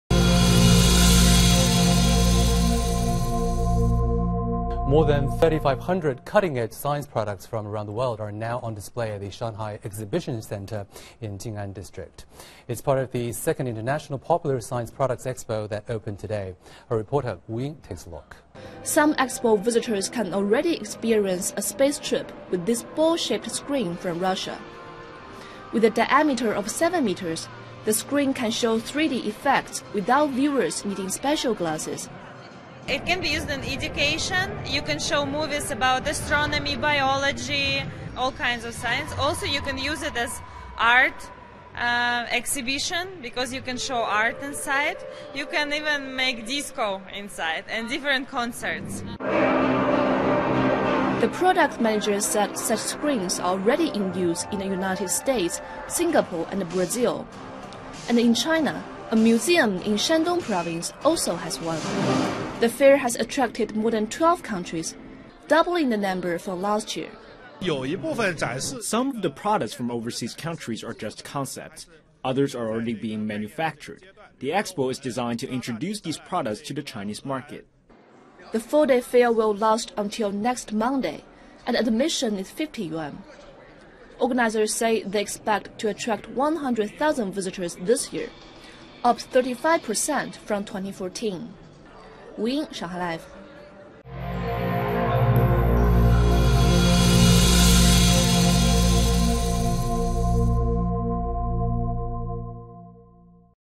Shanghai Science Expo 2015│Reportage by ICS channel.
A large-scale exhibition for the popular science industry was taking a place in Shanghai International Popular Science Products Expo on  November 27-30, 2015.
Fulldome.pro set up there 7-meter dome and our wonderful project-manager Tatiana Retinskaya gave a short interview for ICS channel about spherical video format.